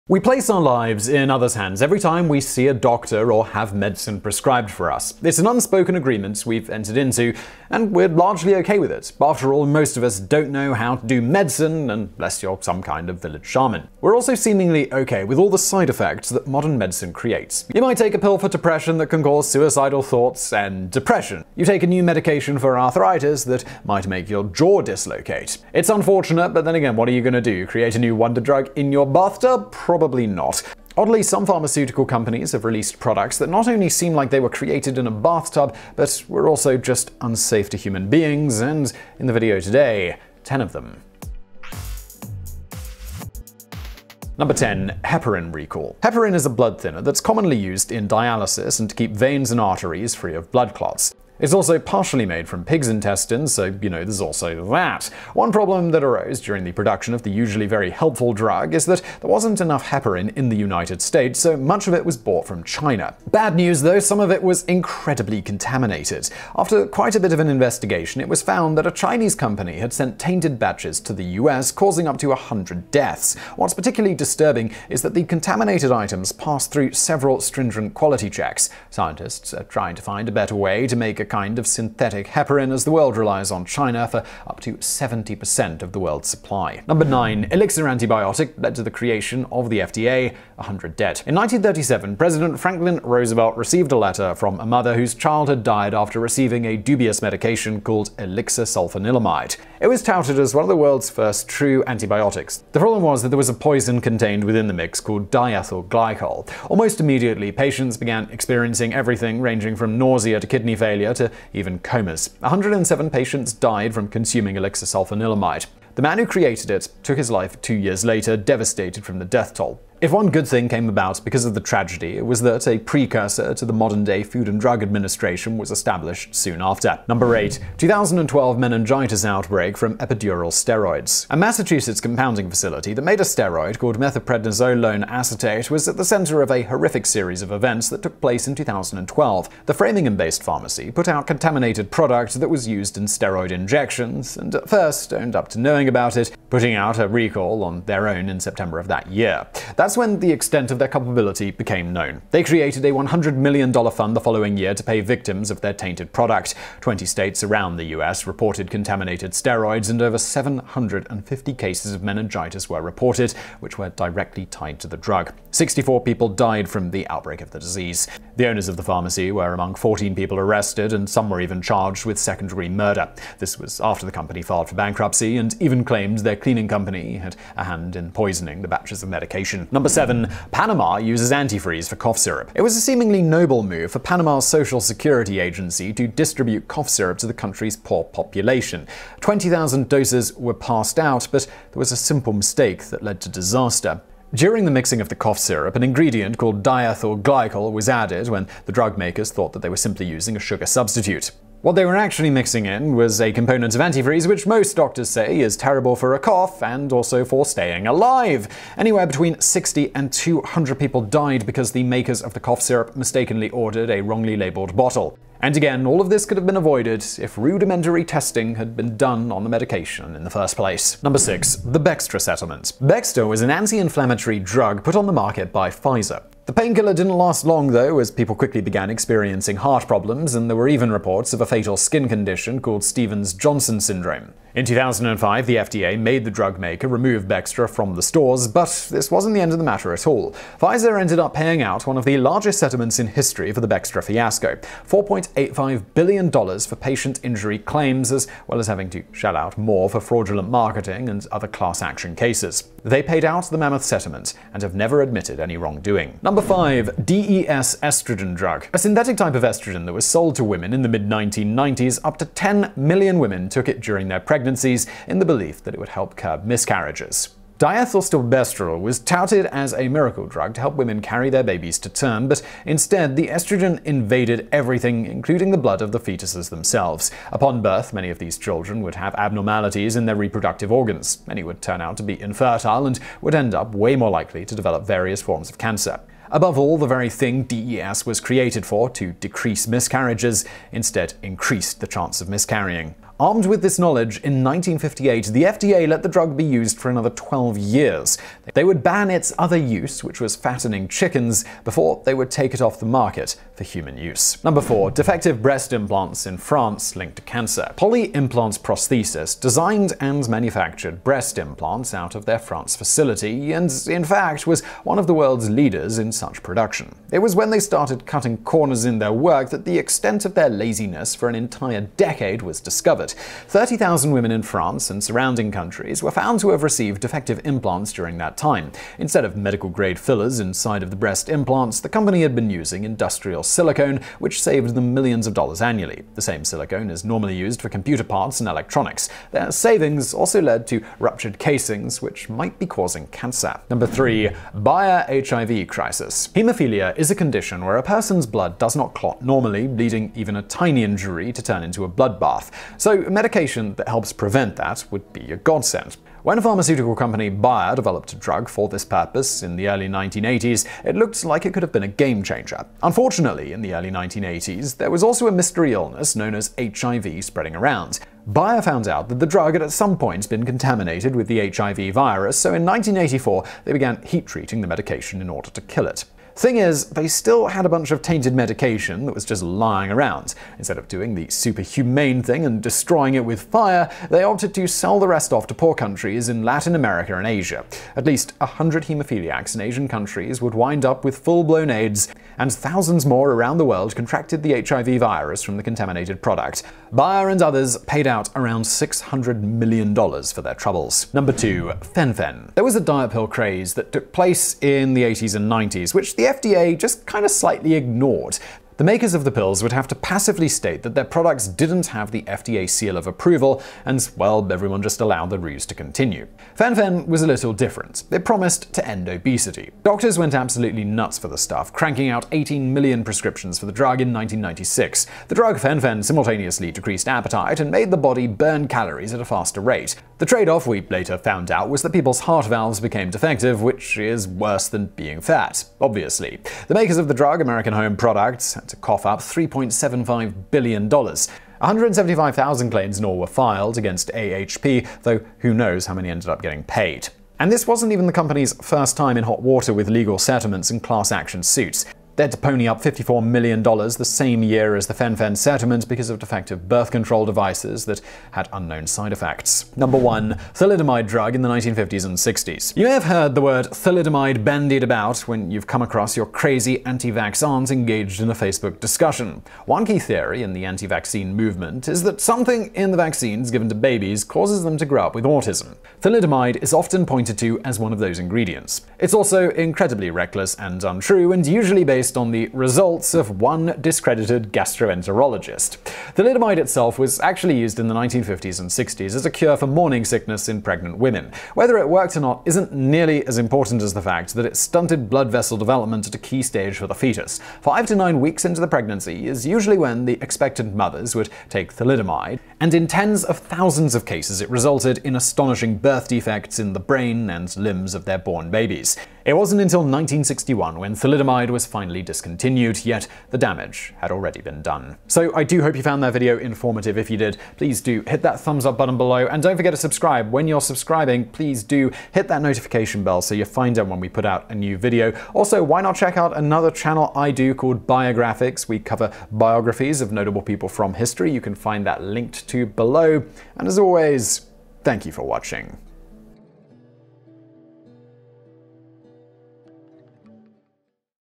10 Catastrophic Failures of the Pharmaceutical Industry
Coming up:

10. Heparin recall
9. Elixir antibiotic (led to creation of FDA) – 100 dead
8. 2012 meningitis outbreak from epidural steroids
7. Panama uses antifreeze for cough syrup
6. The Bextra settlement
5. DES estrogen drug
4. Defective breast implants in France linked to cancer
3. Bayer HIV crisis
2. Fen-Phen
1. Thalidomide drug in 1950s and ’60s